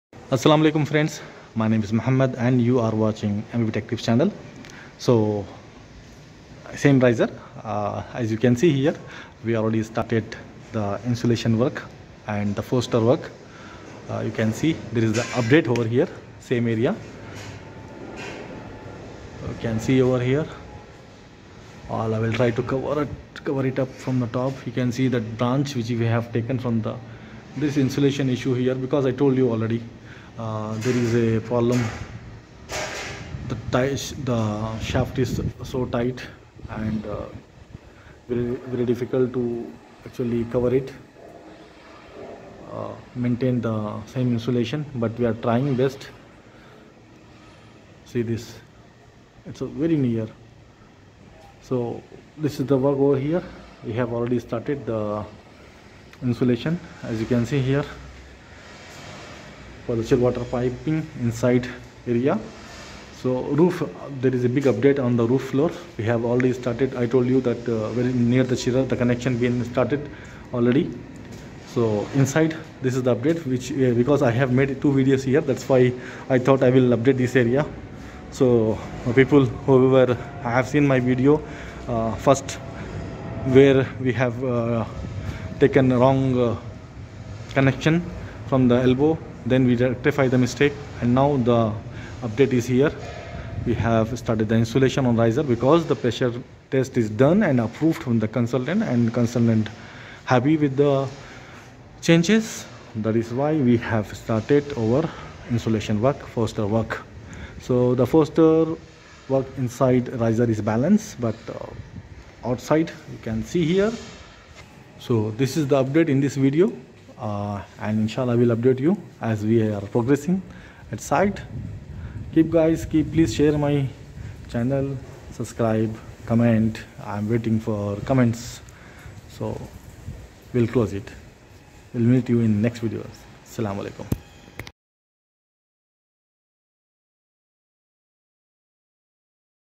Chilled Water Pipe (Update) Insulation work done on Riser inside gas plant room |HVAC by mep techtip
Assalamualikum
Friends

In this video, the same riser where we have done the rectification insulation work is shaft size too tied but still we have done the insulation.

Just check this out

More updates is going to come on roof chiller area.